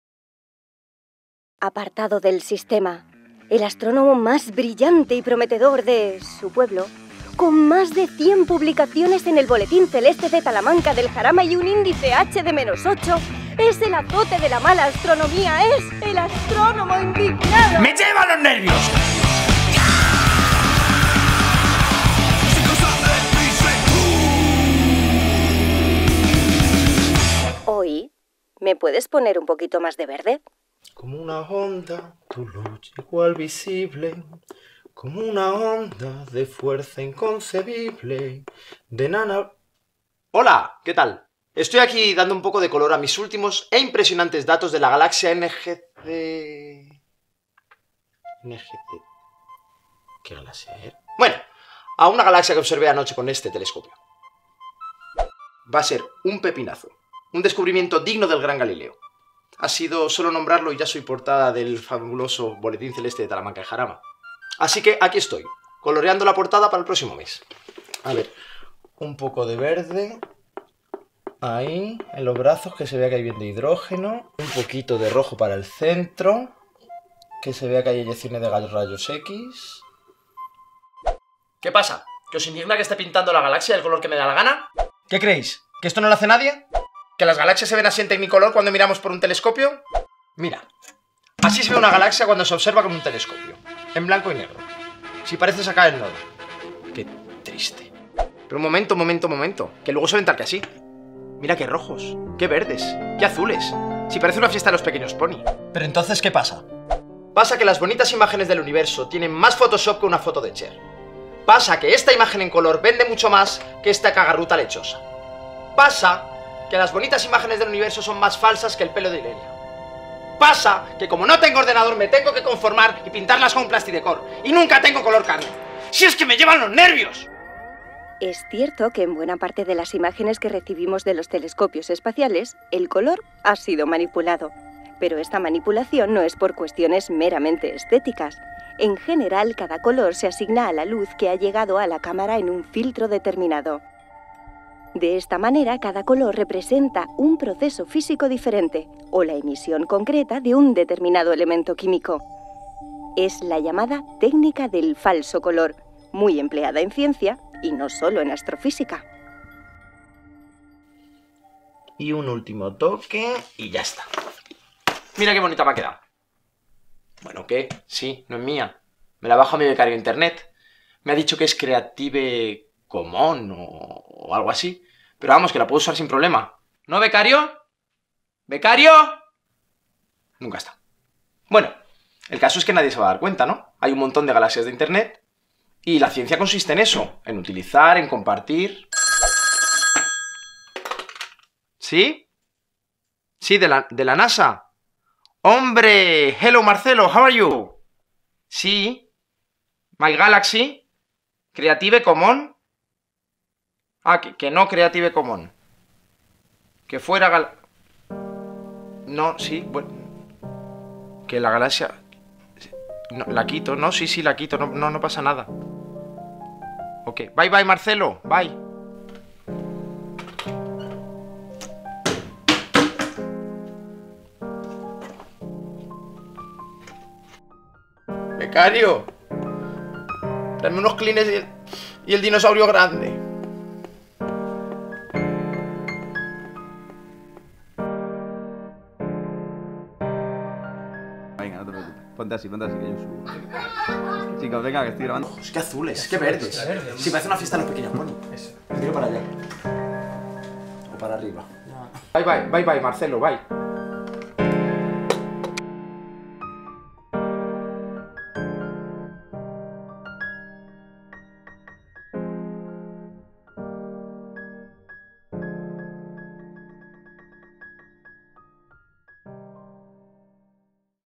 ASTRÓNOMO INDIGNADO IV - ¿ME PONES UN POQUITO MÁS DE VERDE?
Si algo caracteriza a la astrofísica es la belleza y el color de sus imágenes. Pero este color forma parte en muchas ocasiones de un tratamiento posterior con el objetivo no solo de realzar la estetica de las imágenes, sino también científico.